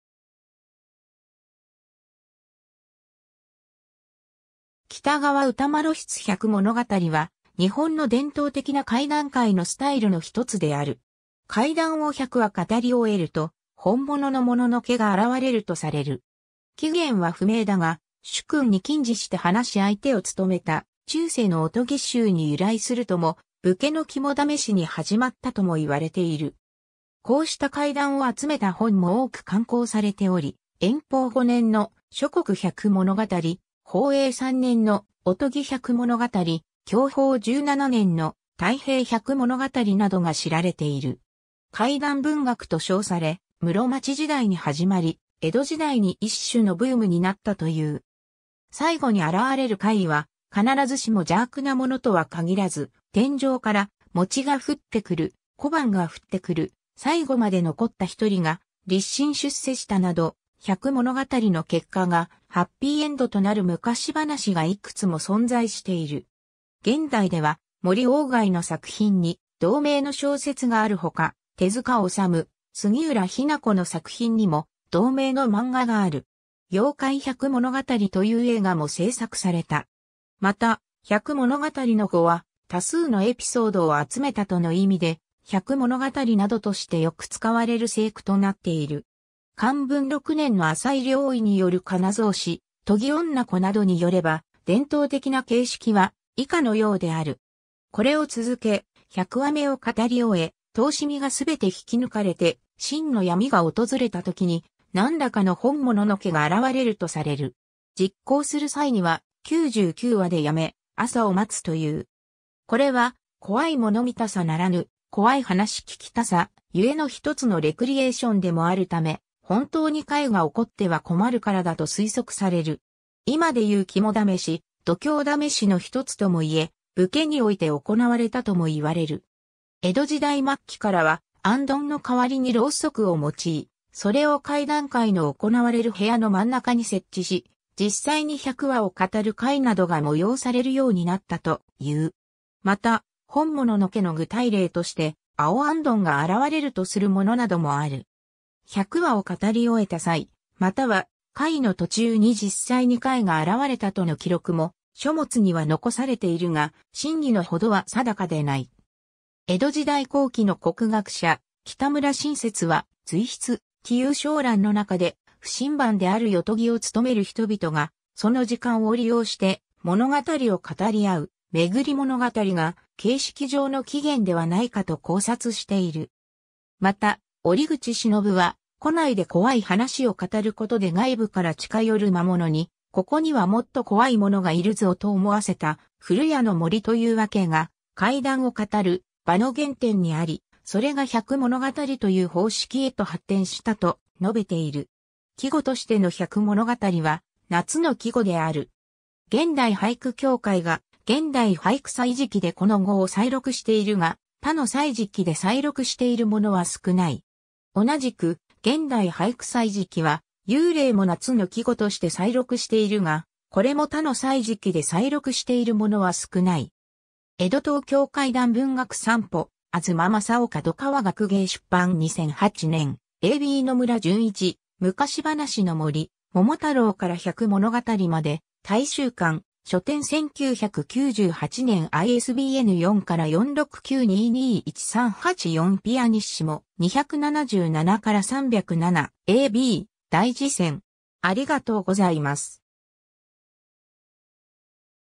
百物語
喜多川歌麿筆 百物語（ひゃくものがたり）は、日本の伝統的な怪談会のスタイルのひとつである。
怪談を100話語り終えると、本物の物の怪が現れるとされる。
起源は不明だが、主君に近侍して話し相手を務めた中世の御伽衆に由来するとも、武家の肝試しに始まったとも言われている。
 こうした怪談を集めた本も多く刊行されており、延宝5年（1677年）の『諸国百物語』、宝永3年（1706年）の『御伽百物語』、享保17年（1732年）の『太平百物語』などが知られている。
怪談文学と称され、室町時代に始まり、江戸時代に一種のブームになったという。
 最期に現れる怪異は必ずしも邪悪なものとは限らず、天井から餅が降ってくる、小判が降ってくる、最期まで残った一人が立身出世したなど、百物語の結果がハッピーエンドとなる昔話がいくつも存在している。
 現代では森鷗外の作品に同名の小説があるほか、手塚治虫、杉浦日向子の作品にも同名の漫画がある。
『妖怪百物語』という映画も製作された。
 また「百物語」の語は、多数のエピソードを集めたとの意味で、「○○百物語」などとしてよく使われる成句となっている。
 寛文6年（1666年）の浅井了意による仮名草子『伽婢子（おとぎぼうこ）』などによれば、伝統的な形式は以下のようである。
 これを続け、100話目を語り終え、灯心がすべて引き抜かれて真の闇が訪れたときに、なんらかの本物の怪が現れるとされる。
 実行する際には、99話でやめ、朝を待つという。
これは「怖いもの見たさ」ならぬ「怖い話聞きたさ」ゆえのひとつのレクリエーションでもあるため、本当に怪がおこっては困るからだと推測される。
今でいう肝試し・度胸試しのひとつともいえ、武家において行われたともいわれる。
 江戸時代末期からは、行灯（灯心）の代わりに蝋燭を用い、それを怪談会の行われる部屋の真ん中に設置し、実際に百話を語る会などが催されるようになったという。
また、本物の怪の具体例として青行燈が現れるとするものなどもある。
100話を語り終えた際、または会の途中に実際に怪が現れたとの記録も書物には残されているが、真偽の程は定かでない。
 江戸時代後期の国学者・喜多村信節は、随筆『嬉遊笑覧』のなかで、不寝番（ふしんばん）である夜伽（よとぎ）を務める人々が、その時間を利用して物語を語り合う「巡（めぐ）り物語」が、形式上の起源ではないかと考察している。
 また、折口信夫は、戸内で怖い話を語ることで外部から近寄る魔物に「ここにはもっと怖いものがいるぞ」と思わせた『古屋のもり』という話型が、怪談を語る場の原点にあり、それが百物語という方式へと発展したと述べている。
 季語としての百物語（ひゃくものがたり）は、夏の季語である。
現代俳句協会が『現代俳句歳時記』でこの語を採録しているが、他の歳時記で採録しているものは少ない。
 同じく、『現代俳句歳時記』は「幽霊（ゆうれい）」も夏の季語として採録しているが、これも他の歳時記で採録しているものは少ない。
 ^ 『江戸東京怪談文学散歩』東雅夫　角川学芸出版 2008年 ^ a b 野村純一『昔話の森：桃太郎から百物語まで』 大修館書店 1998年 ISBN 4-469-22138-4 pp.277-307. ^ a b 『大辞泉』